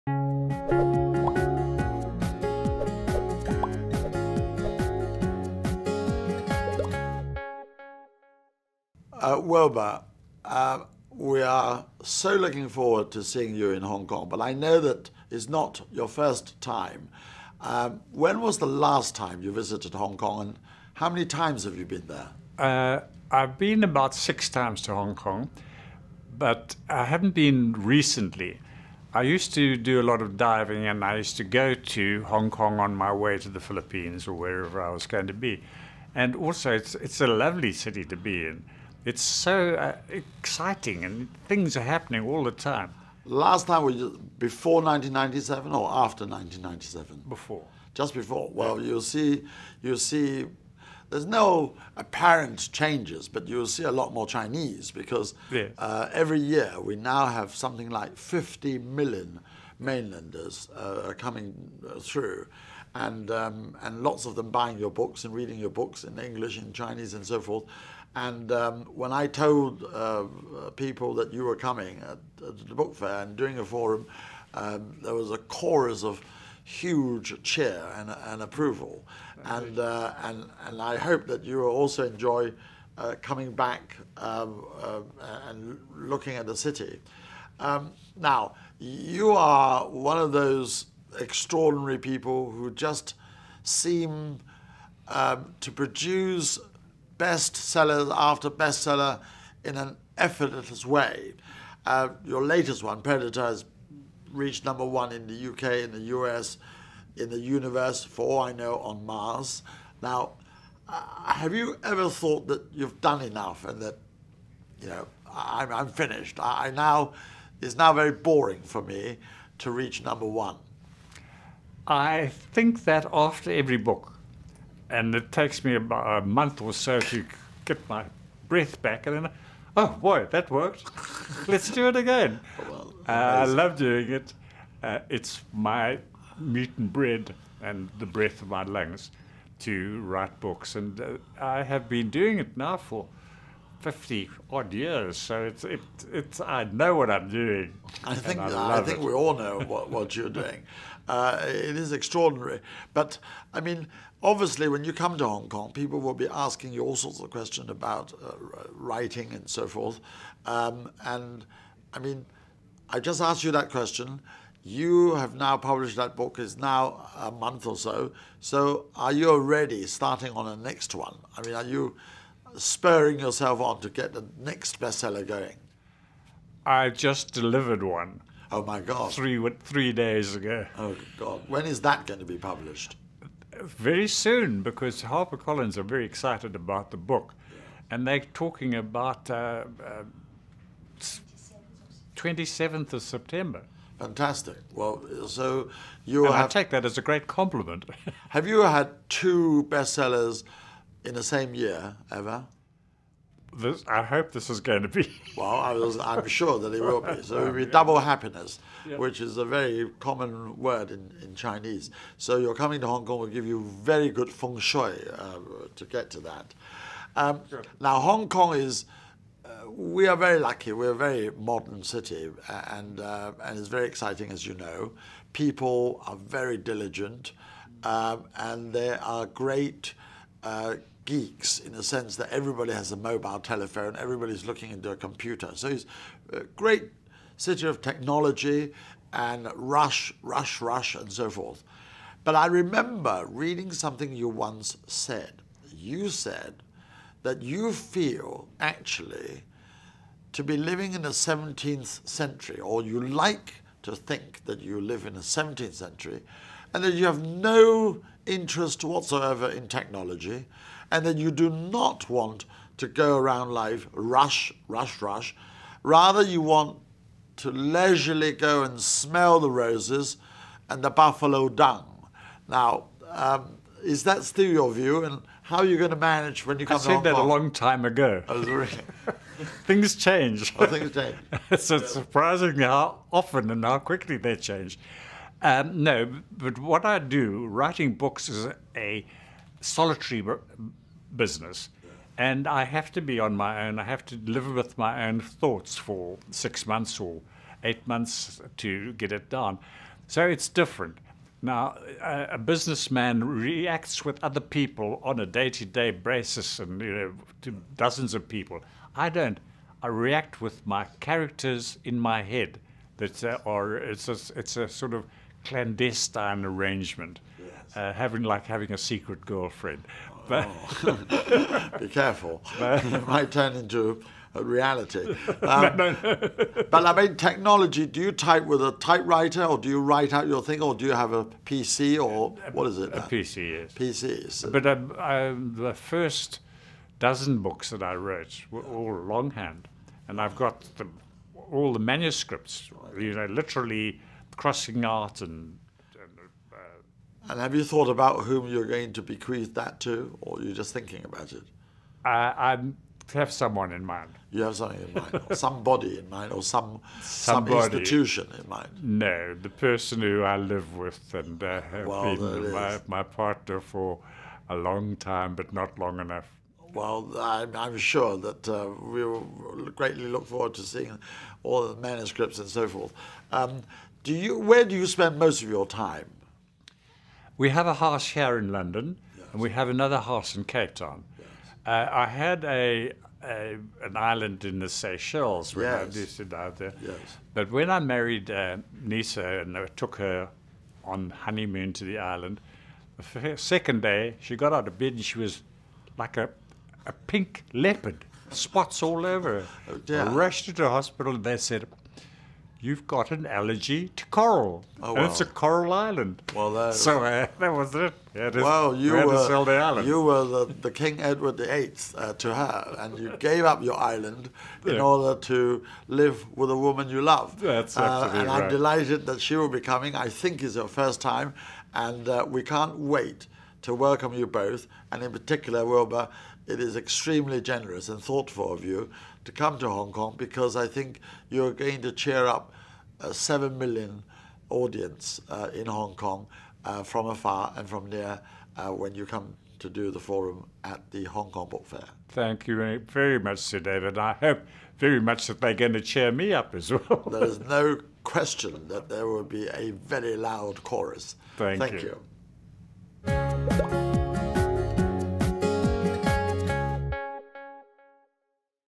Wilbur Smith: Writing a “Solitary Business” – Hong Kong Book Fair 2016
Author and journalist Wilbur Smith describes writing as “a solitary business”  to create characters in his historical fiction works. Mr Smith spoke with Sir David Tang for this interview ahead of their participation in the 22 July 2016 Open Public Forum with fellow writers Simon Winchester and Hannah Rothschild. The Forum is part of the HKTDC Hong Kong Book Fair - 20-26 July at the Hong Kong Convention and Exhibition Centre.